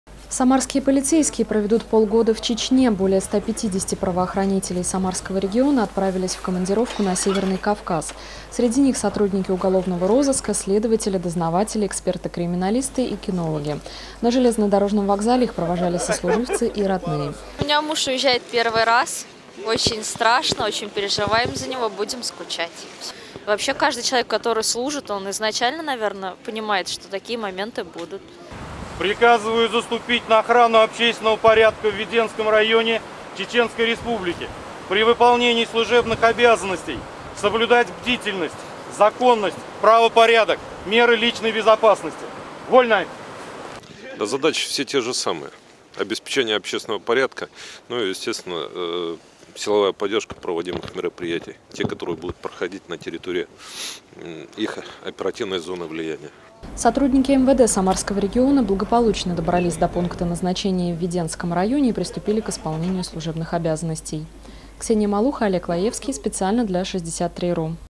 Самарская полиция в Чечне
Более 150 сотрудников МВД России по Самарской области приступили к исполнению служебных обязанностей в месте временной дислокации в Веденском районе Чеченской республики.